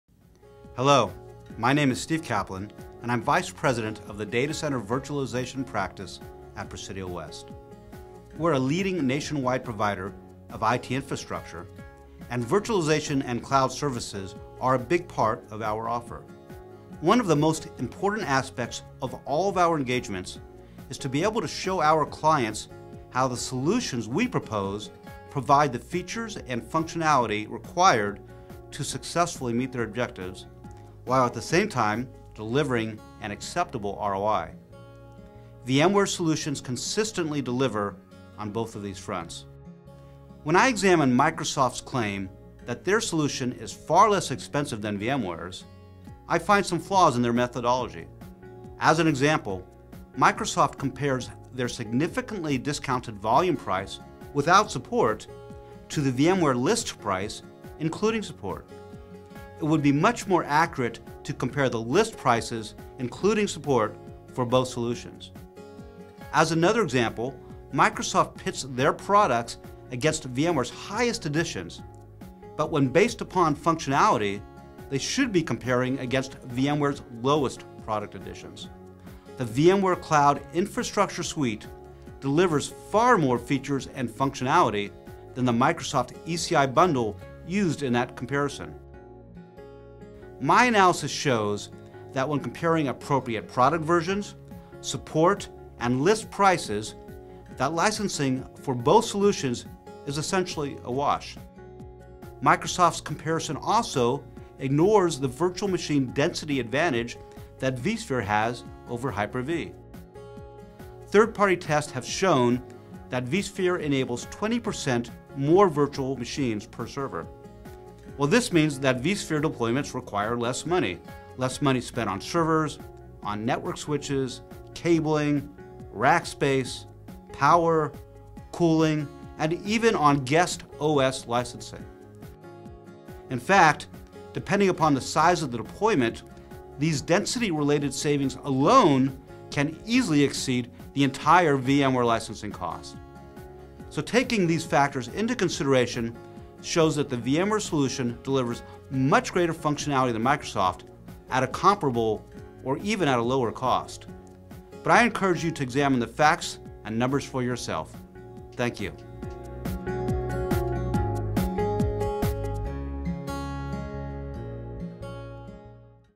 VMware vSphere Delivers Greater Value and Lower TCO than Microsoft Hyper-V
-- Get the Facts, Not the Microsoft Hyper-bole
"VMware delivers far greater value than Microsoft"
-- Steve Kaplan, Vice President, Presidio